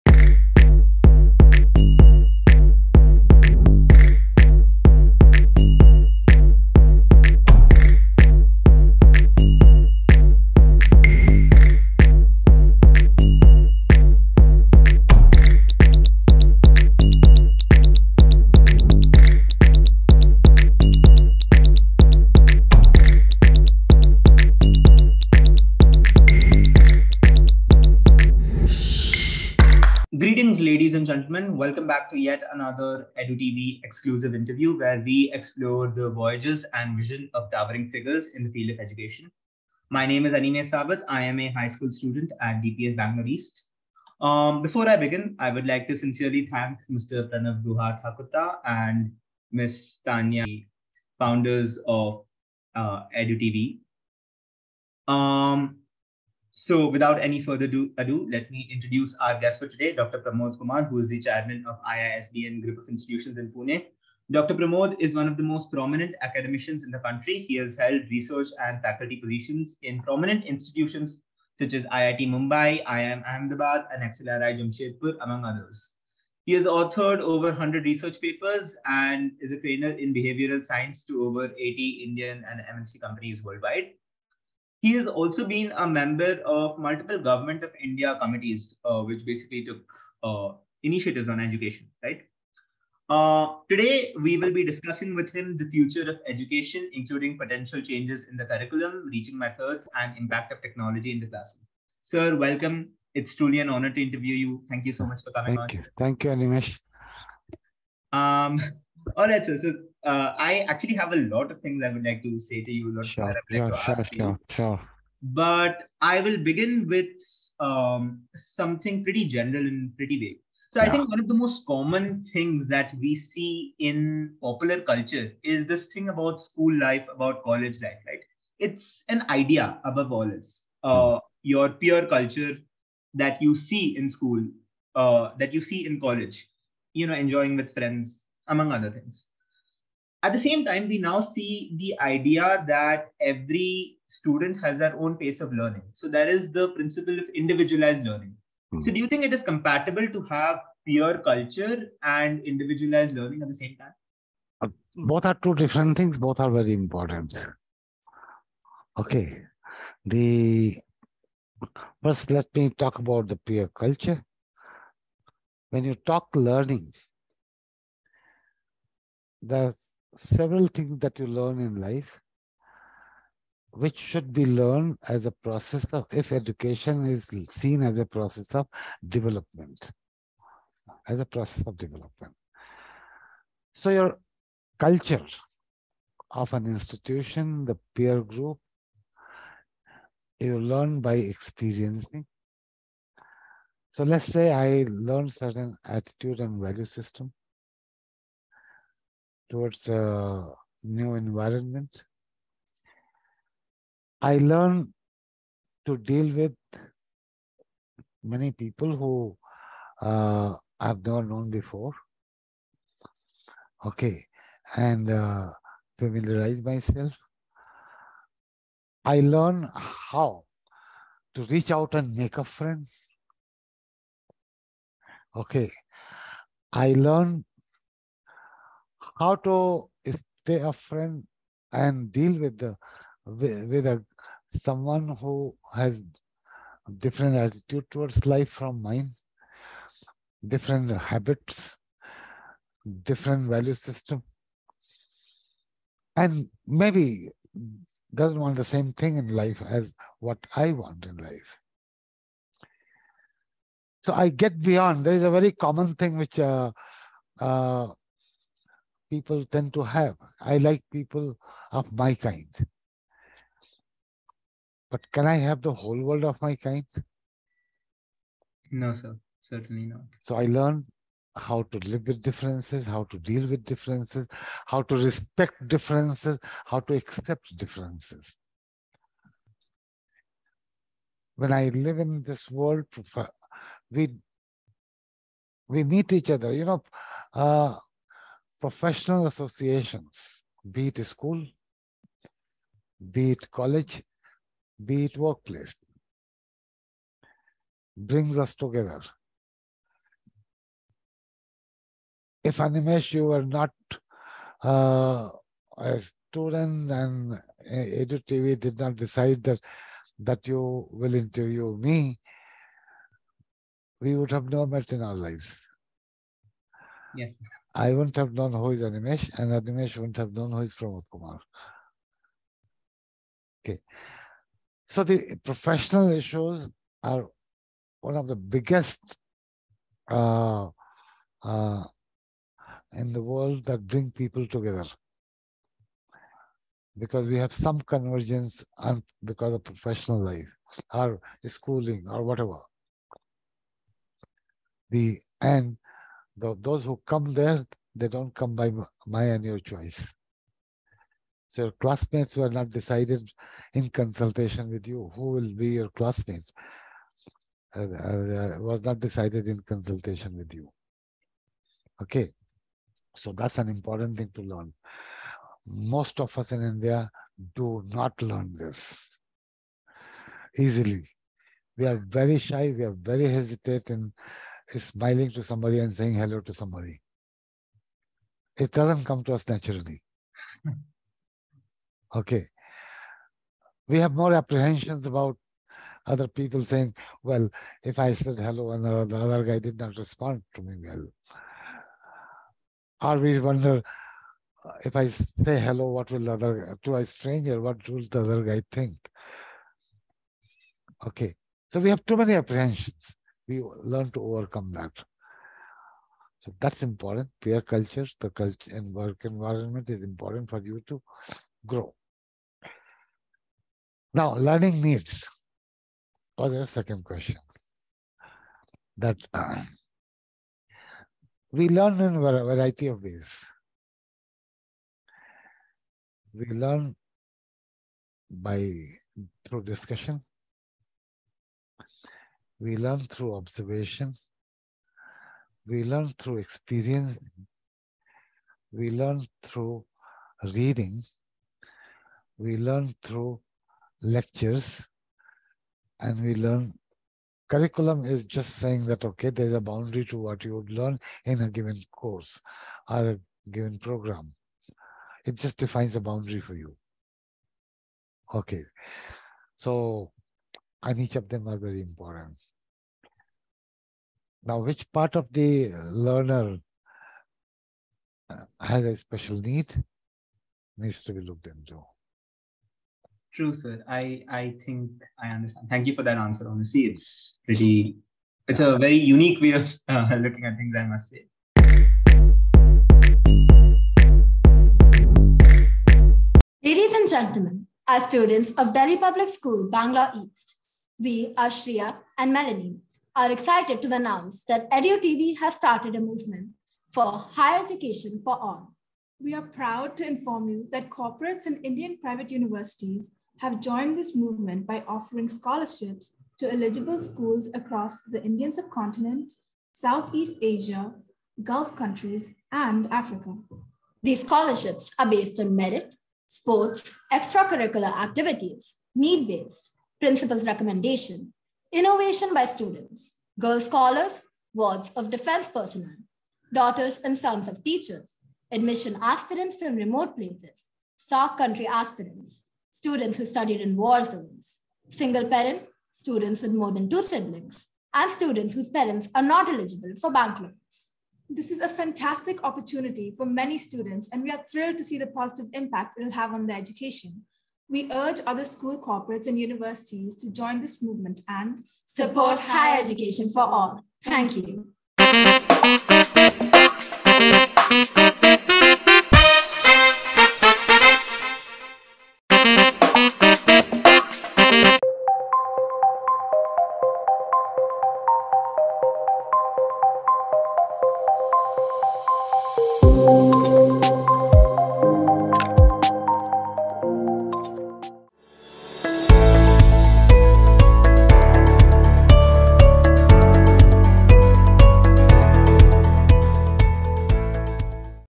Dr Pramod Kumar, ISB&M India President, speaks on EDU TV to DPS Bangalore East student
About Dr. Pramod Kumar
DECISIVE MOMENTS AND ACTIONS SHAPE YOUR EDUCATION AT ISB&M
Dr. Pramod Kumar,
President

Ph.D. (Organizational Behavior), IIT Mumbai, Formerly with IIM Ahmedabad, Former Chairperson Placement, XLRI, Jamshedpur; Ex-Director, Symbiosis Institute of Business Management, Pune; Consultant to over 80 companies worldwide. Author of over 100 research papers, cases and management games. Research quoted internationally in textbooks and journals. Served on Government of India Committees on Management Education.